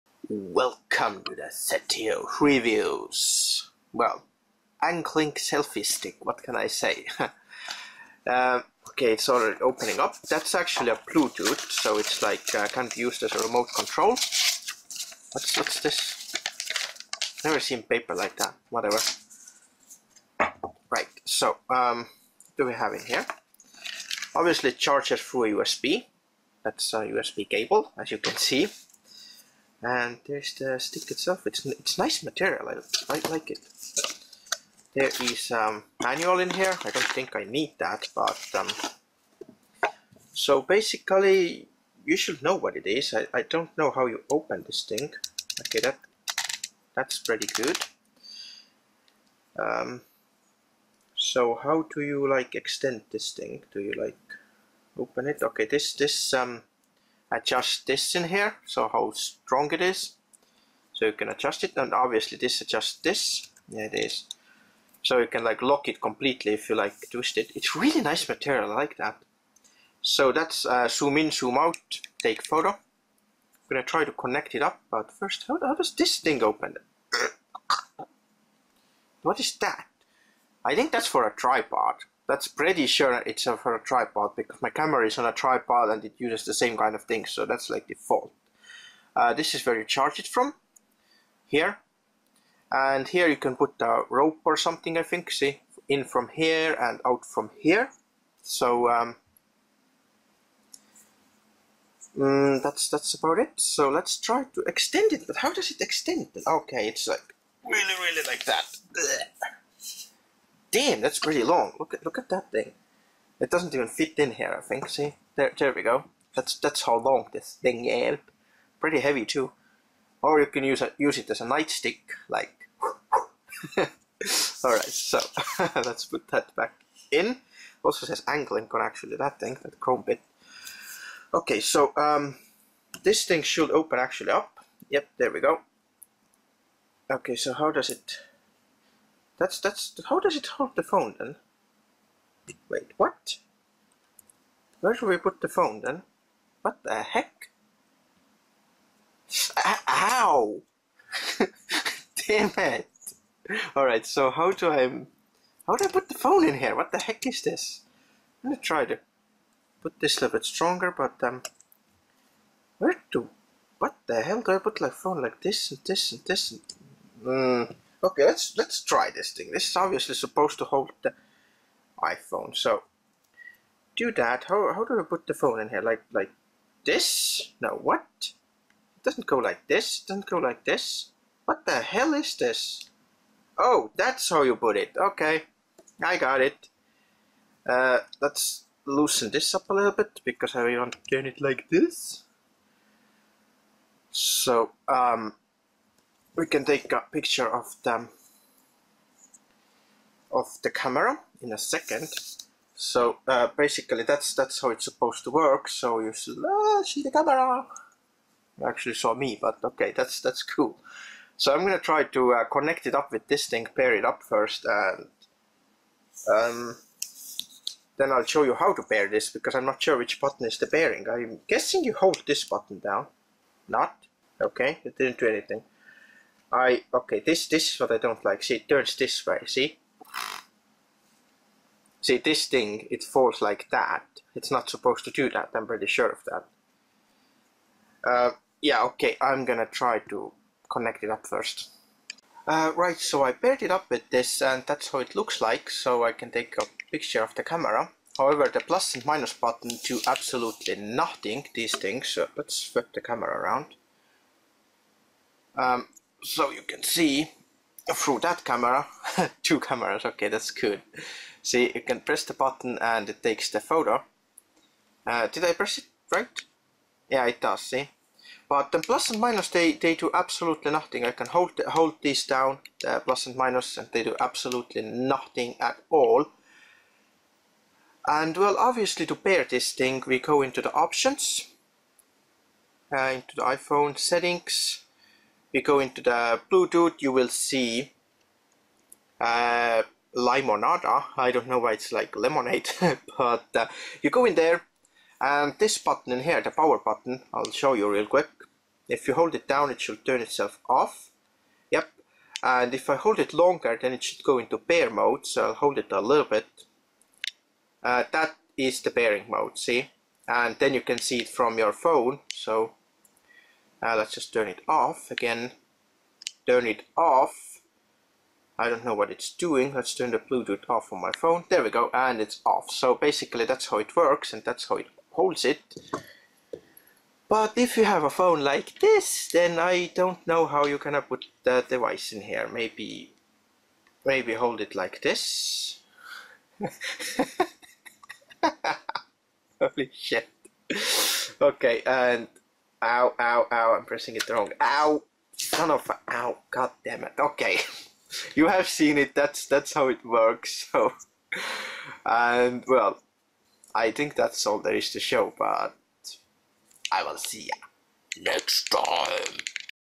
AngLink Bluetooth Selfie Stick REVIEW
Well it's a selfie stick .. with bluetooth! You can use it as remote trigger too.